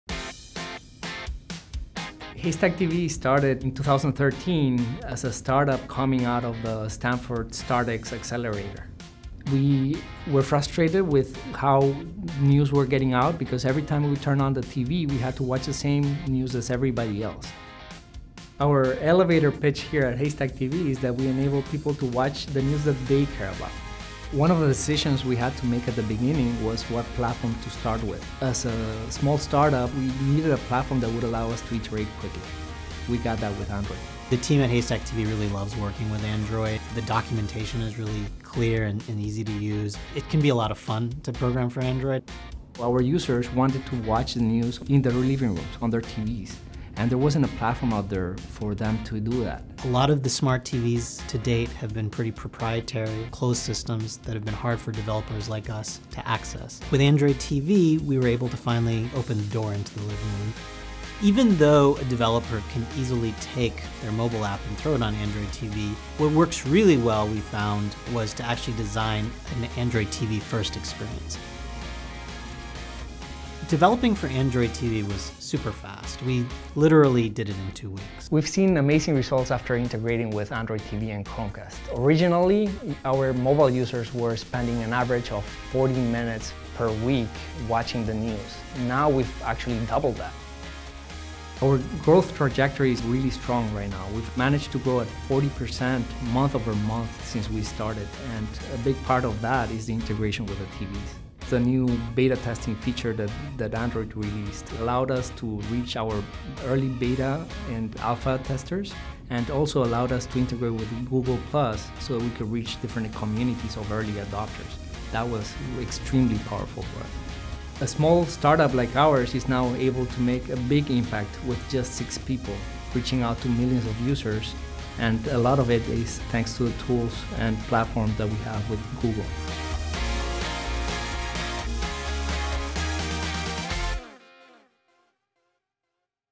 Android Developer Story: Haystack TV Doubles Engagement with Android TV
Haystack TV built a scalable business with six employees and Android TV. Two weeks was all it took for them to bring their mobile app to the big screen. That includes building an immersive, cinematic UI (a task greatly simplified by the Android framework). Watch how they grew their weekly average viewing time from 40 minutes to 80. Now, their business is growing at 40% per month. You can too.

To get more tips, check out The Secrets to App Success on Google Play [ebook]